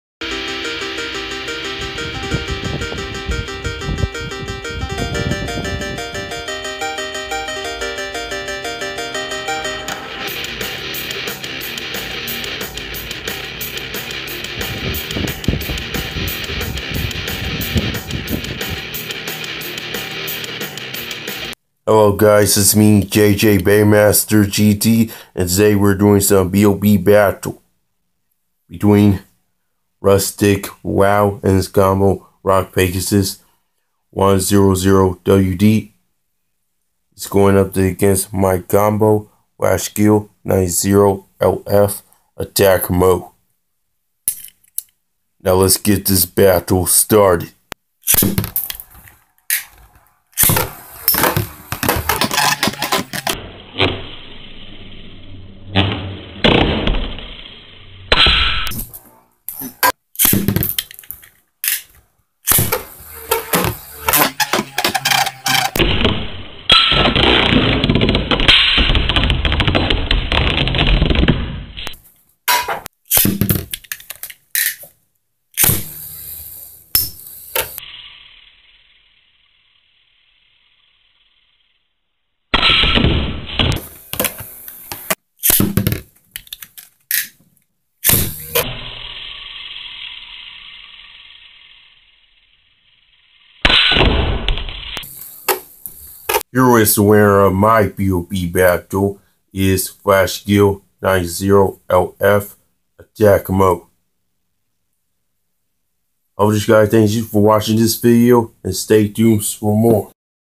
B.O.B Rustiq Wow Vs JJ BeyMaster GT
Battle of Bladers going up to against Rustiq Wow!

Enjoy watching this and stay toons for more videos of beyblade battles.